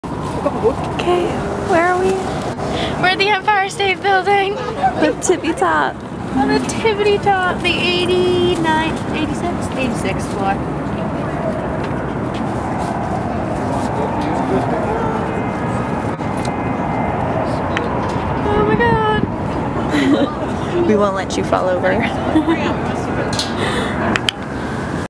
Great heights - Empire State Building view
View from the top of the Empire State Building with Caroline